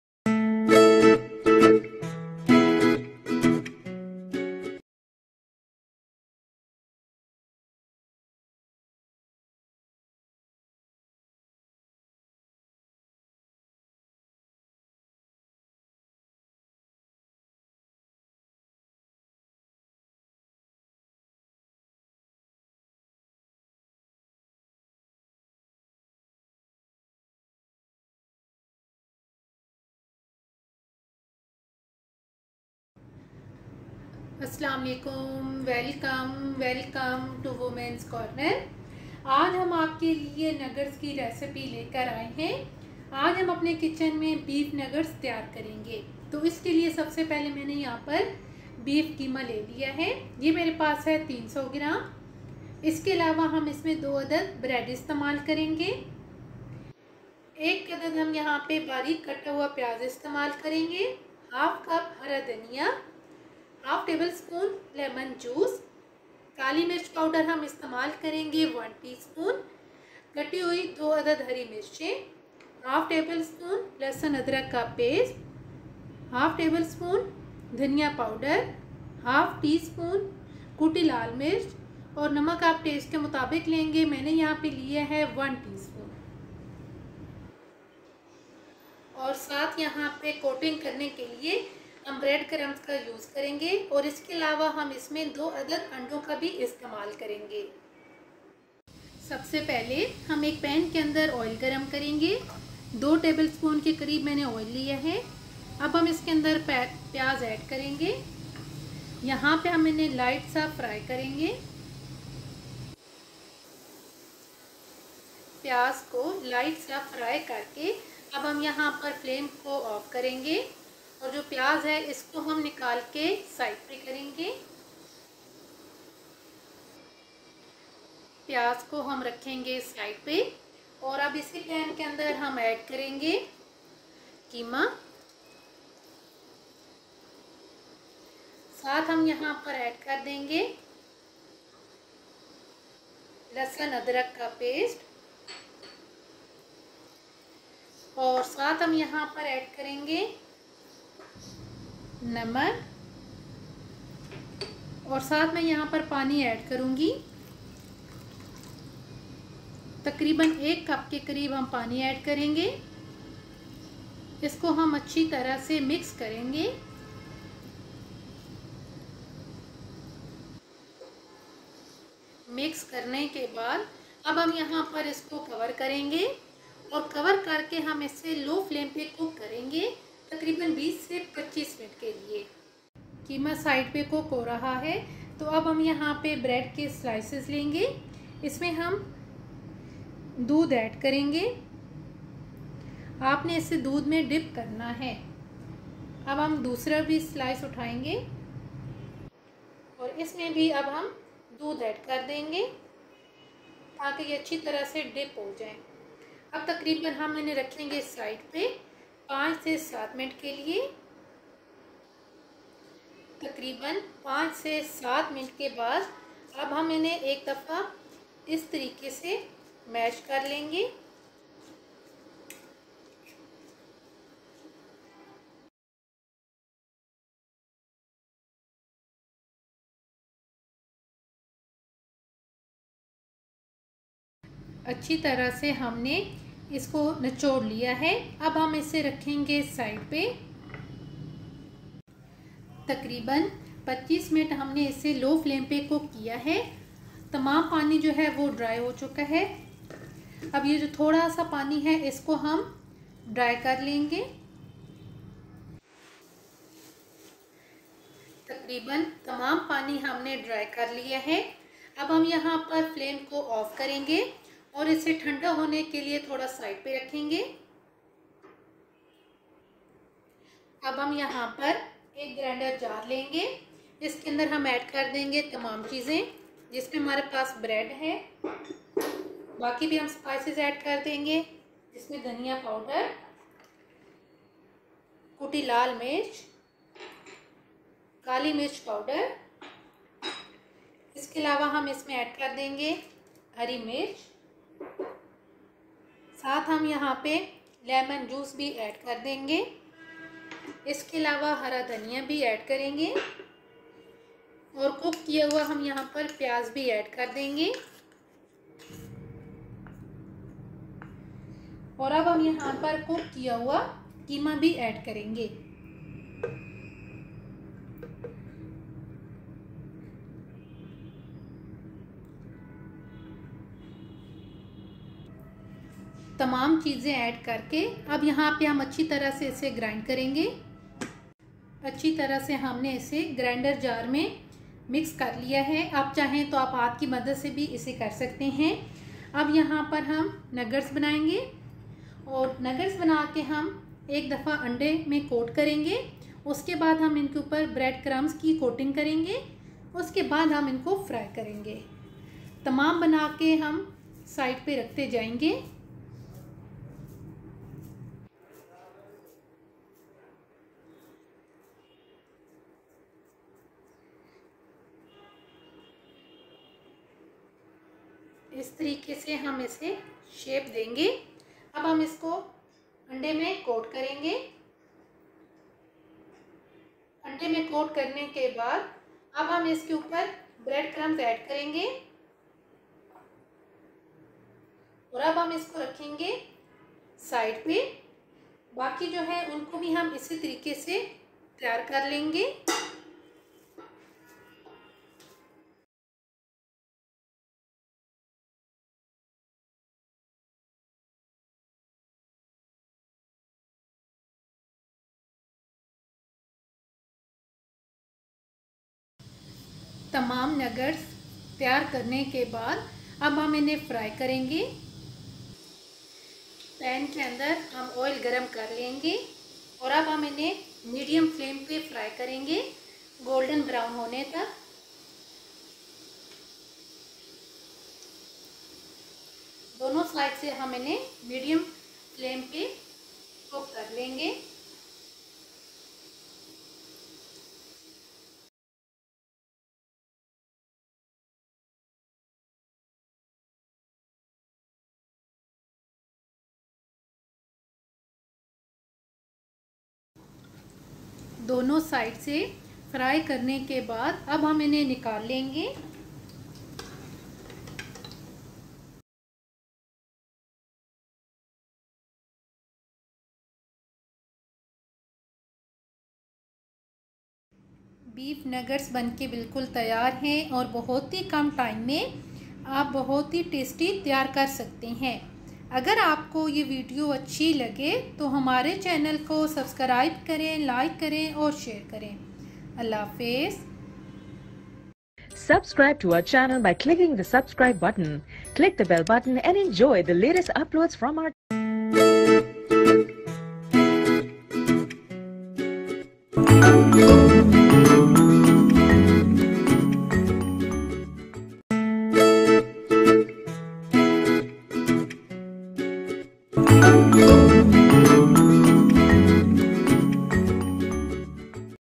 beef nuggets | beef nuggets recipe in urdu | how to make beef nuggets | Women's corner
Tasty food is that which makes you to close your eyes, lean back, and whisper 'yesssss.'
Here in this video I will tell you about beefnuggets recipe that you can easily follow to cook a delicious beefnuggets dish for your family.

Keep watching .